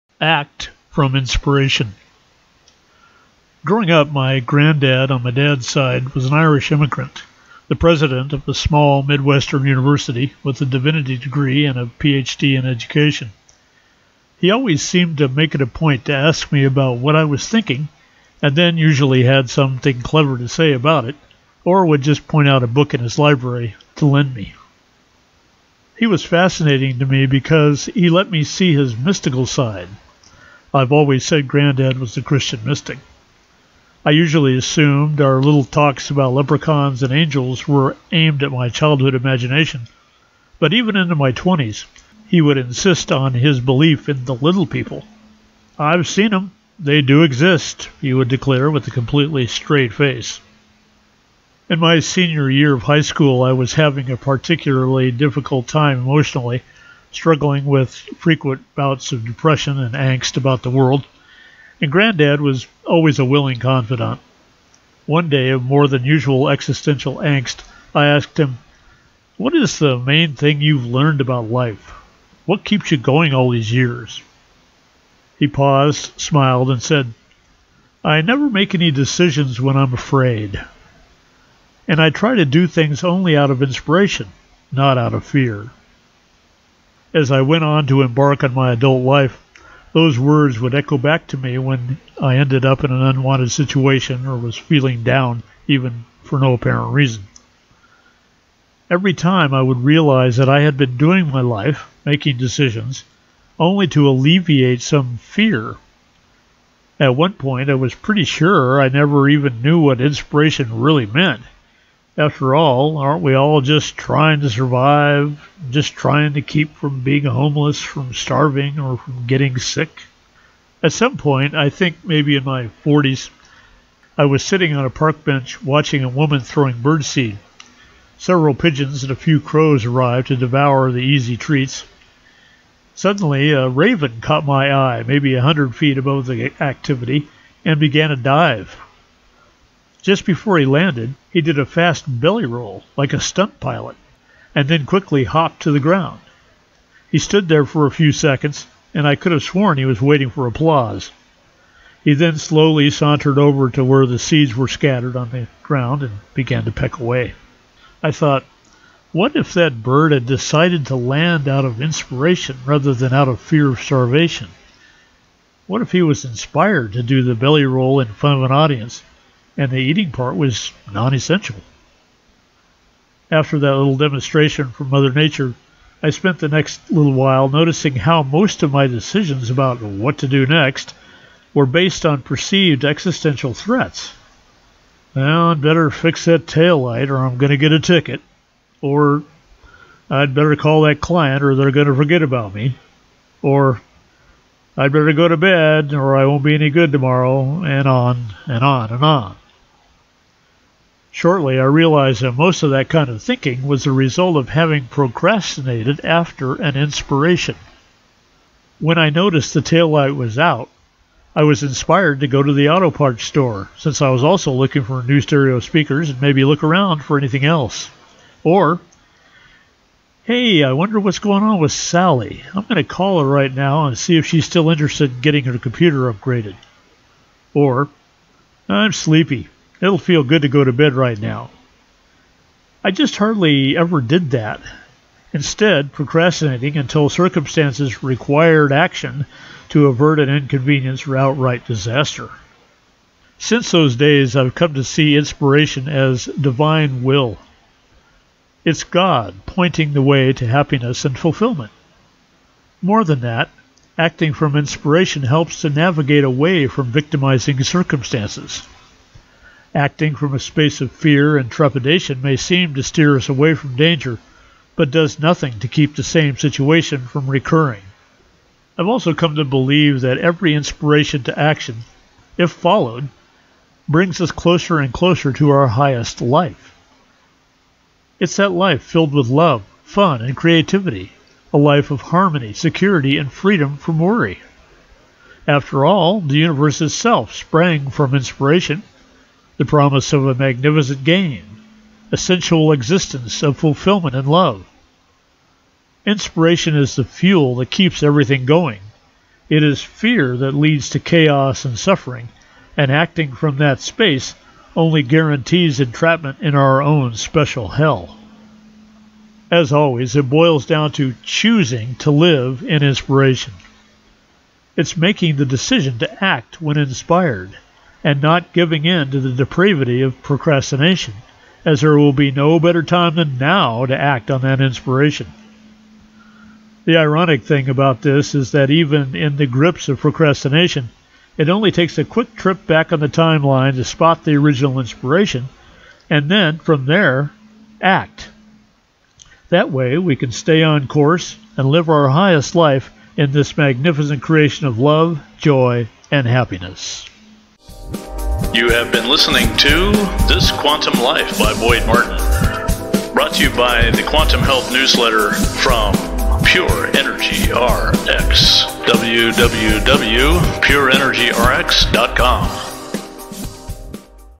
This Quantum Life #121 - Act From Inspiration
I've come to believe that every inspiration to action, if followed, brings us closer and closer to our highest life.